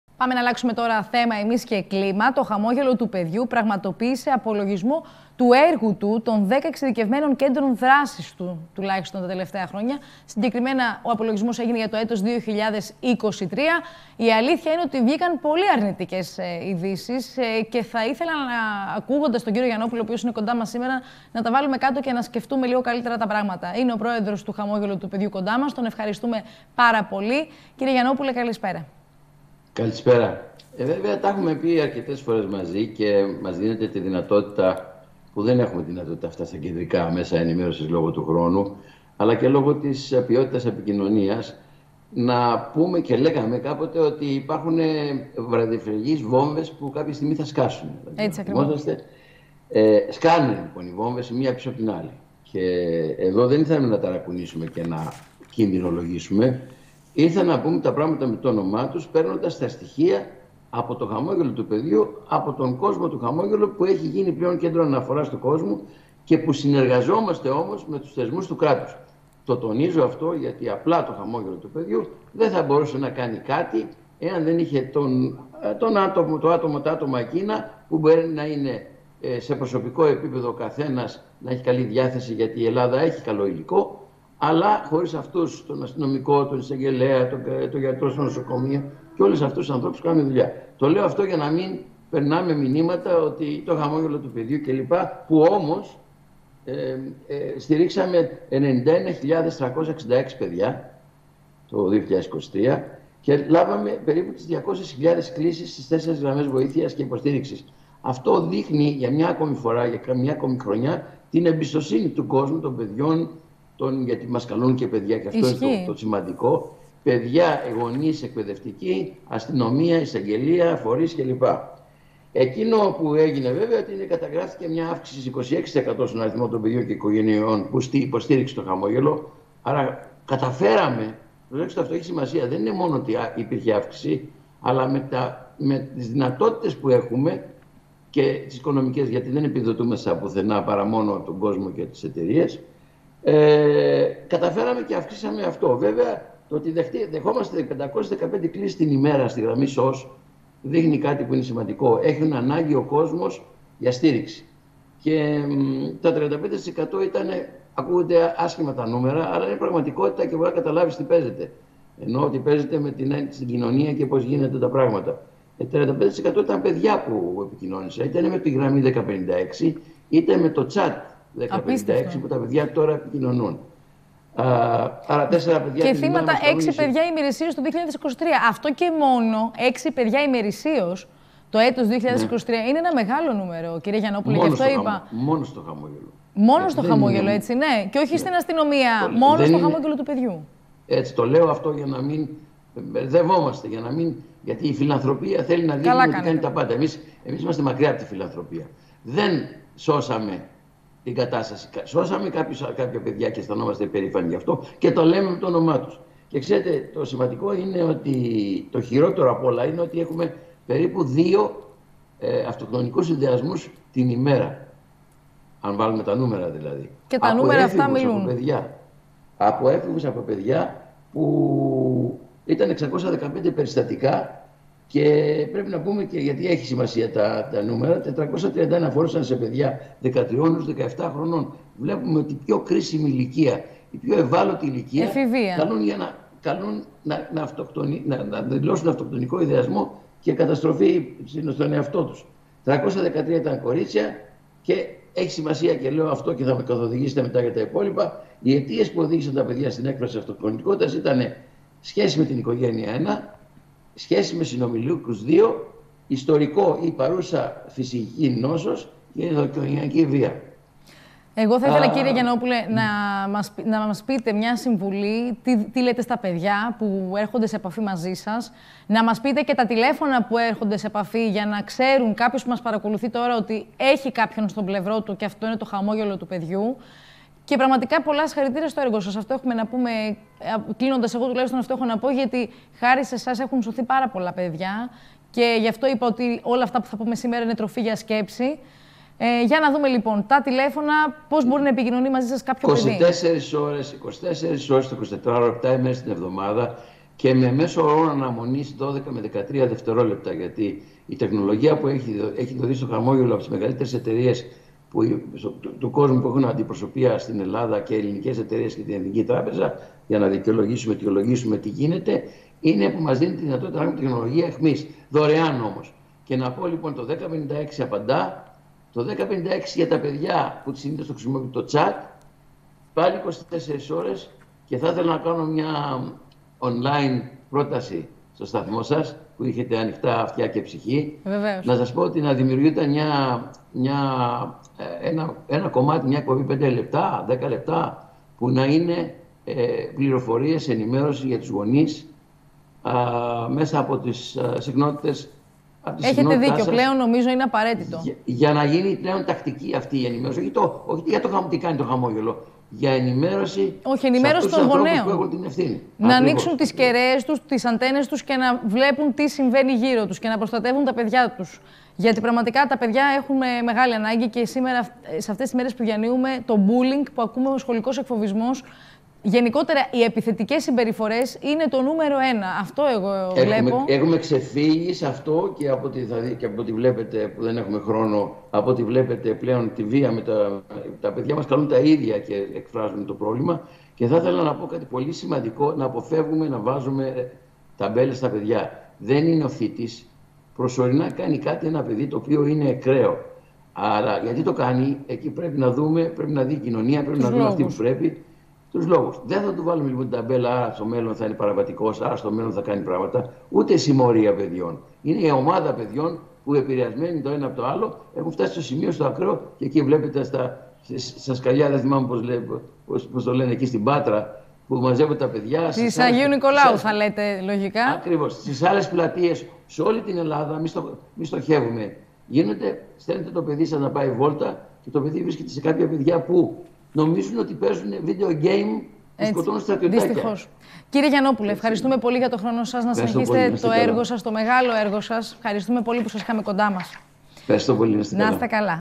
Θύματα κακοποίησης έξι παιδιά ημερησίως το 2023
Έξι παιδιά την ημέρα έπεφταν θύματα κακοποίησης, ενώ σχεδόν δύο παιδιά την ημέρα, εμφάνιζαν αυτοκτονικές τάσεις μέσα στο 2023, σύμφωνα με στοιχεία που ανακοίνωσε νωρίτερα σήμερα το «Χαμόγελο του Παιδιού».